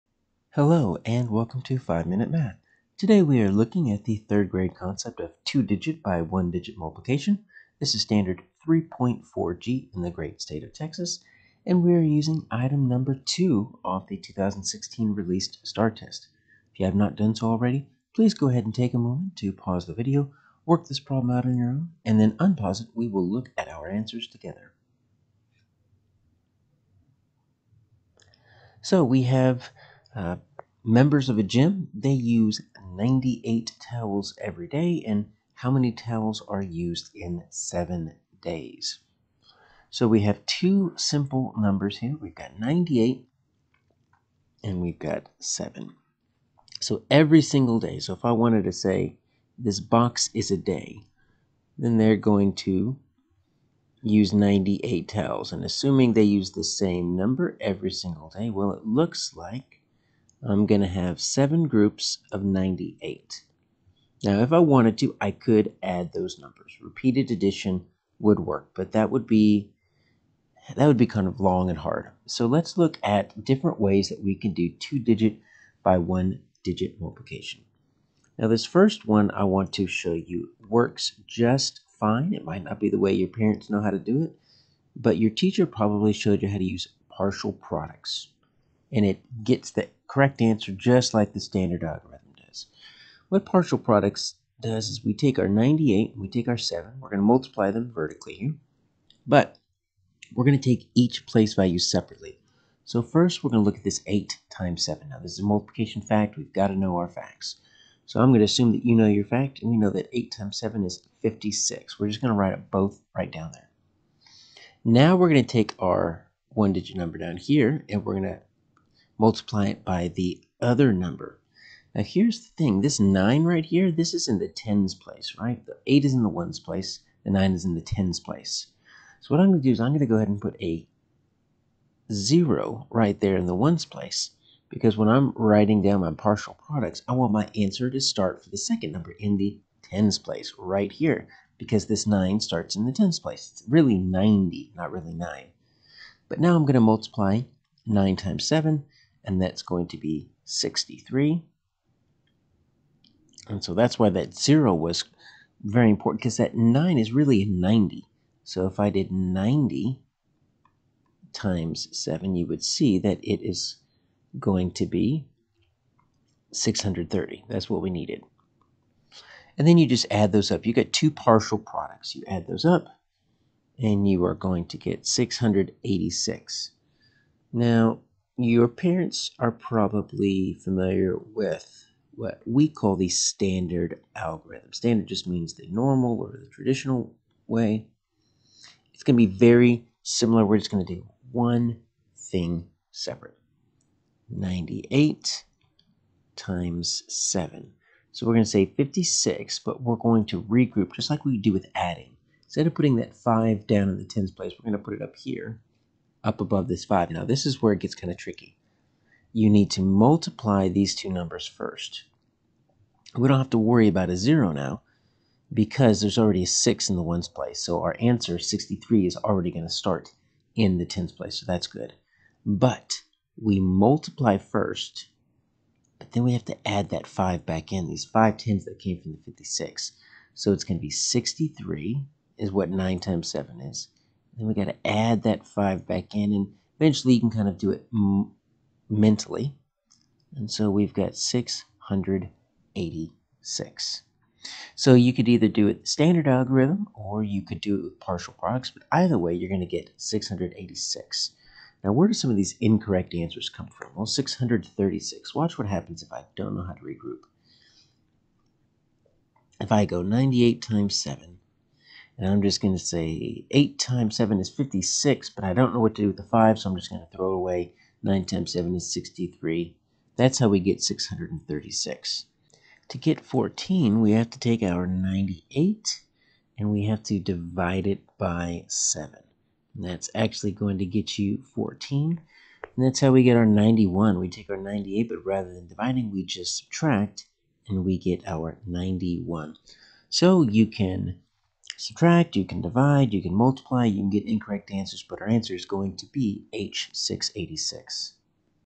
3rd Grade STAAR Practice 2-Digit by 1-Digit Multiplication (3.4G - #1)
Practice using strategies and algorithms, including the standard algorithm, to multiply a two-digit number by a one-digit number. Strategies may include mental math, partial products, and the commutative, associative, and distributive properties with a test item from the 2016 release STAAR test (#2 - 3.4G). I explain the correct answer and why the incorrect answers are wrong.